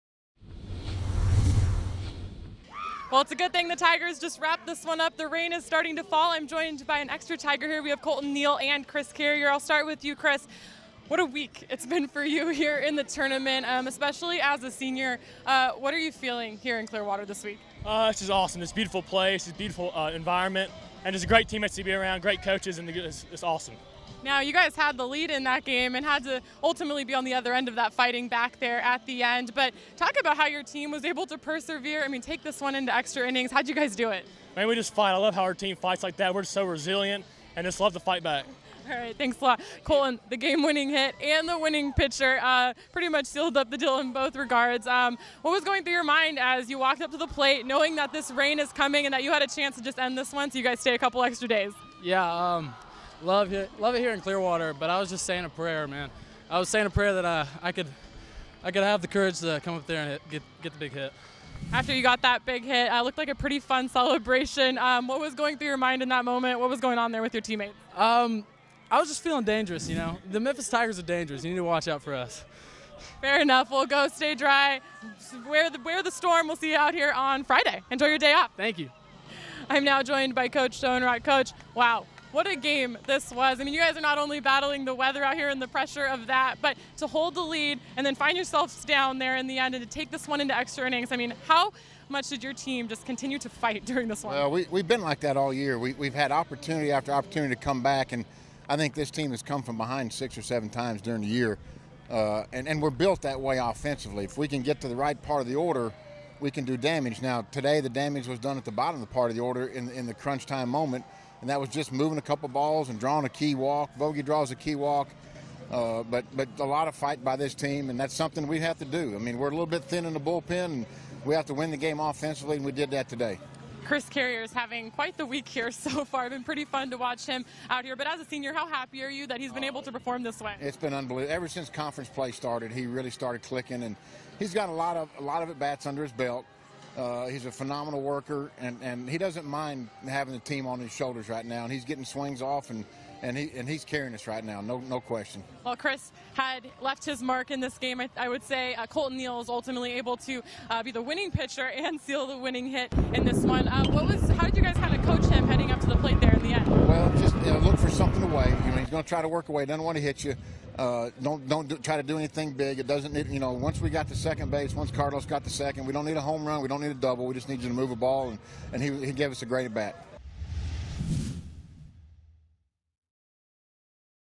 2017 American Baseball Championship Day 2 - Memphis Postgame Interviews
🚶OFF ALERT!!!

In the game that set the conference tournament record for the most combined runs scored, the Memphis Tigers​ came back from a SIX RUN deficit to beat the Cincinnati Bearcats in extra innings and avoid elimination. Hear from Head Coach Daron Schoenrock as well as Senior Chris Carrier and the hero Colton Neel.